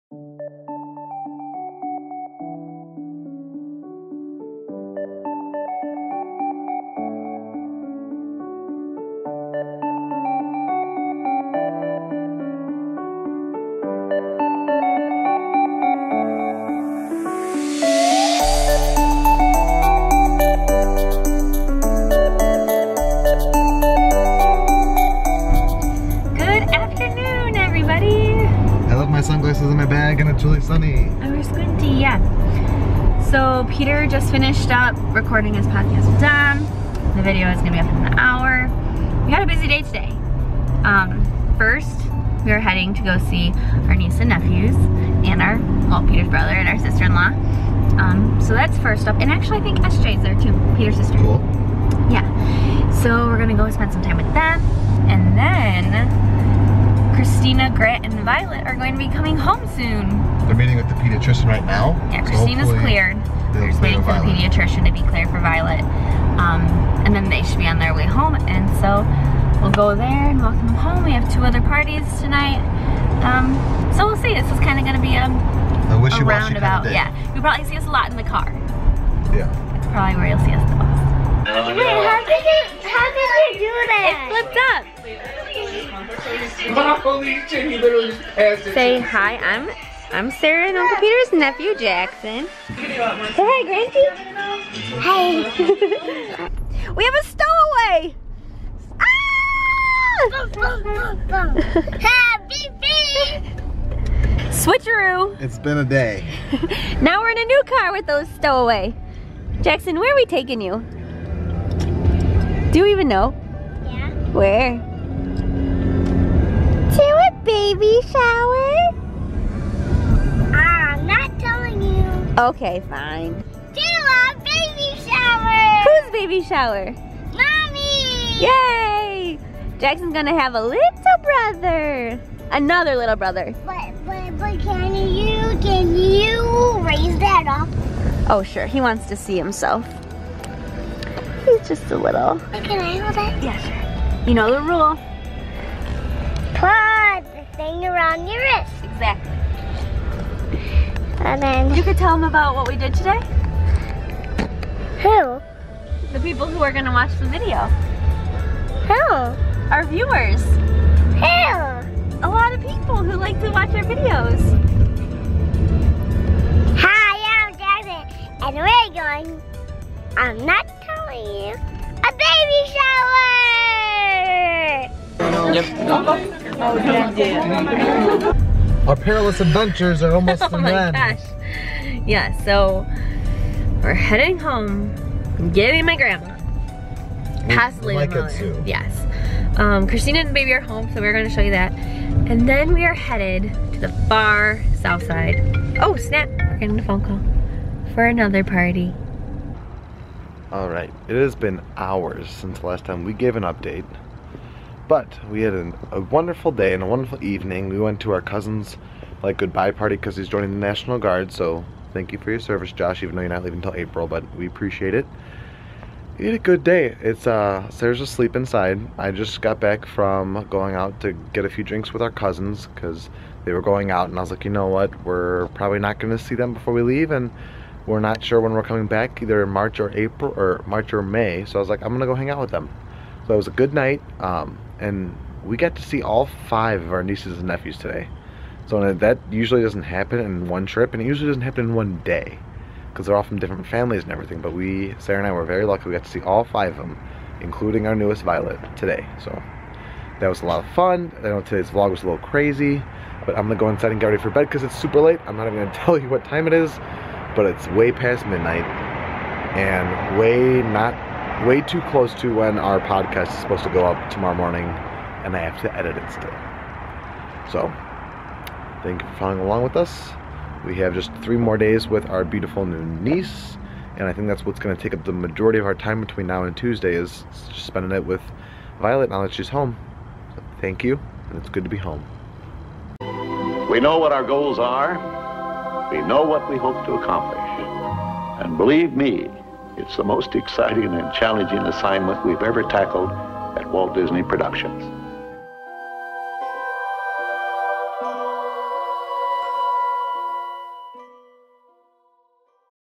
Nephew Vlog Takeover | Nonstop Family Parties
We had a busy busy day running around to see all of our nieces and nephews along with a baby shower and a farewell party for our cousin. Peter recorded the next installment of the Park Hopper Podcast with Dom.